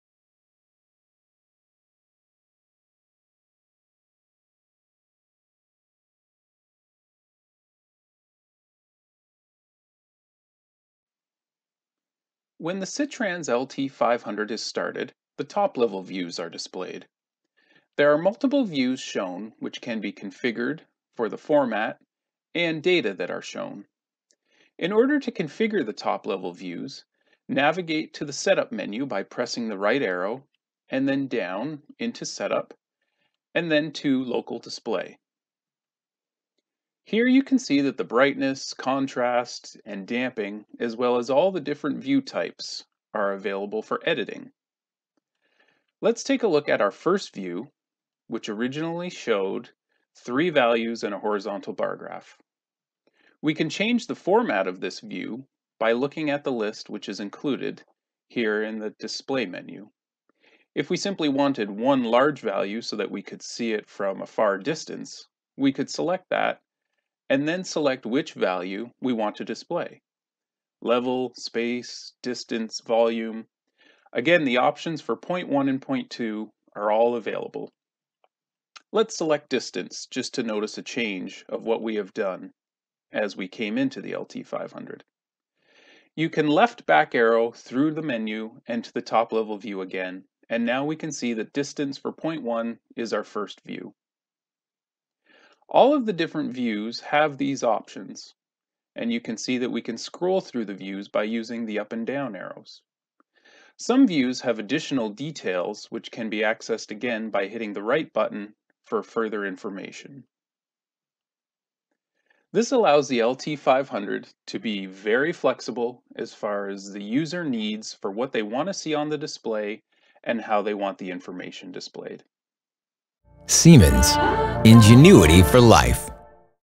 SITRANS LT500 - Easy view setups with Matt Morrissey
Need flexibility in your display settings? SITRANS LT500 provides 6 different display screens with 6 different view types which can display relay status, digital inputs, process value bar graphs, diagnostics and 12 different PV options along with totalizers!